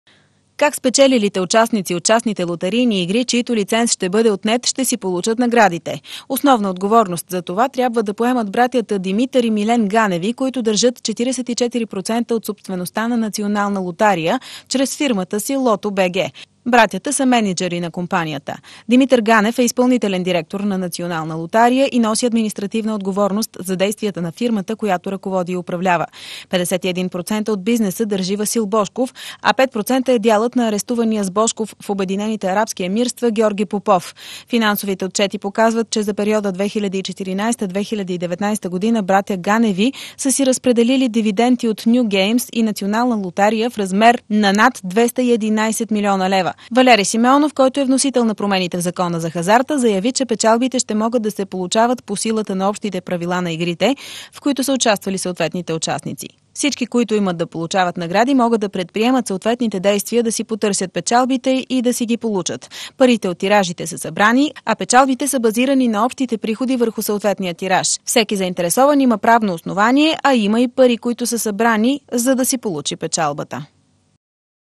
Има как спечелилите от частните лотарии да си получат наградите?
Братята Димитър и Милен Ганев са бизнесмените, които трябва да поемат отговорност по изплащането на печалбите от частните лотарийни игри, чийто лиценз предстои да бъде отнет